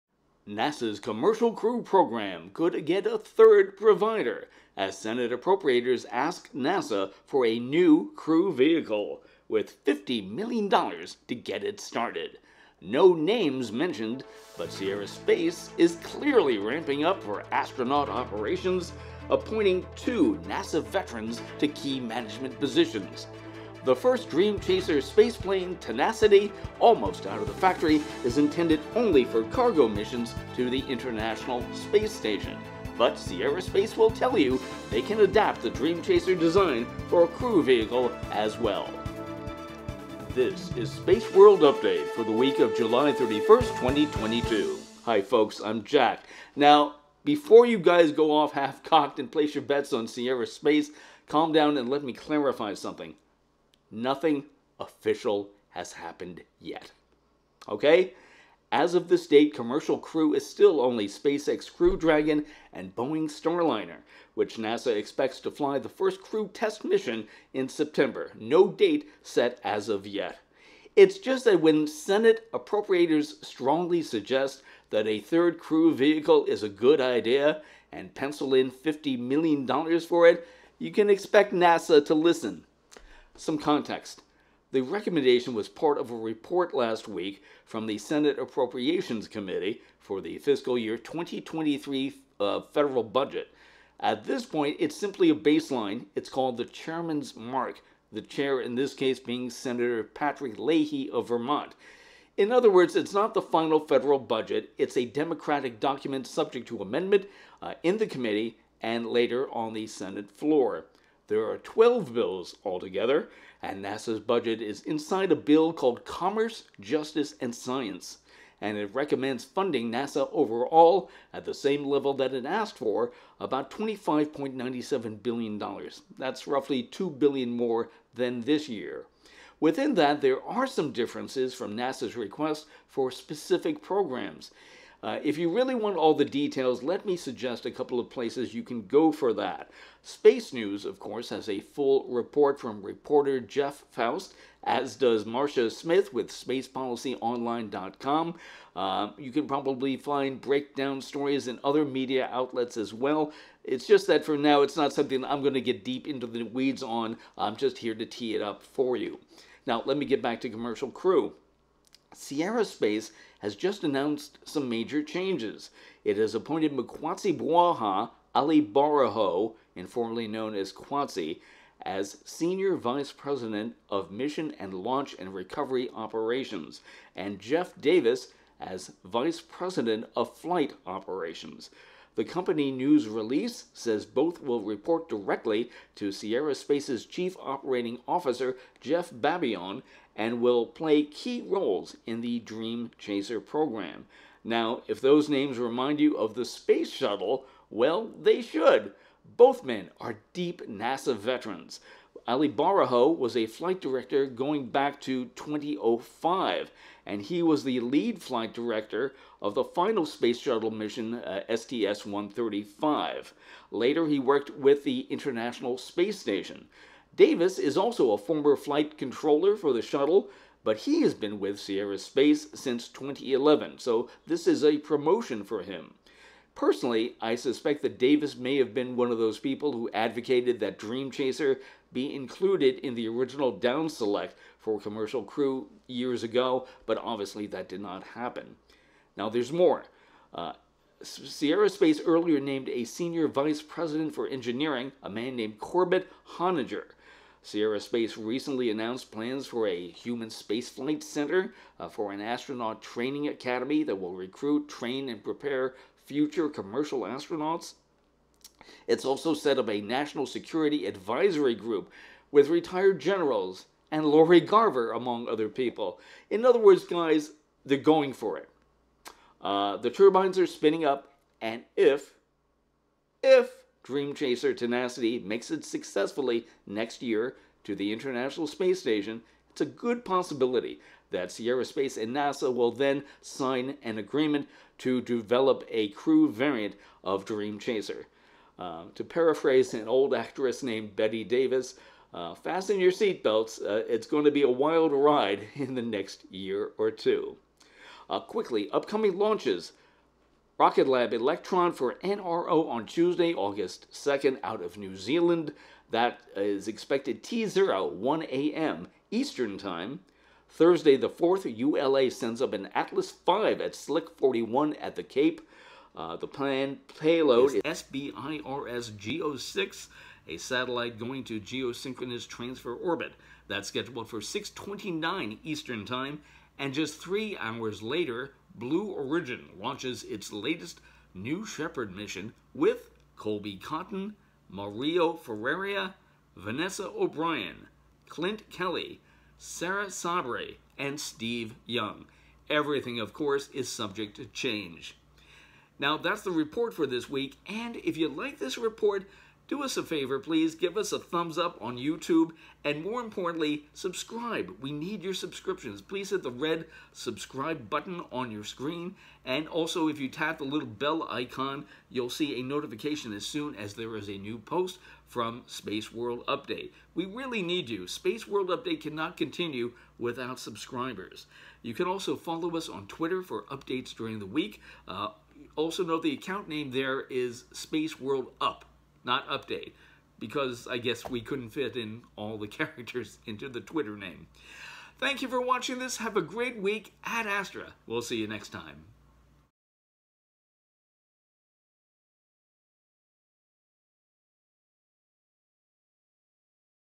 Sierra Space Aims for Commercial Crew! (Space World Update 31 July 2022)
Sierra Space of Colorado is aiming to join Boeing and SpaceX in the NASA Commercial Crew program. The SierraSpace Dream Chaser spaceplane named Tenacity is already expected to soon become the new cargo vehicle going to the ISS. However, DreamChaser may also become a future commercial crew vehicle as well. Something important has just happened that may make NASA more interested in having a third provider for astronaut transport to the International Space Station.

Also, some brief discussion of the FY 2023 NASA budget.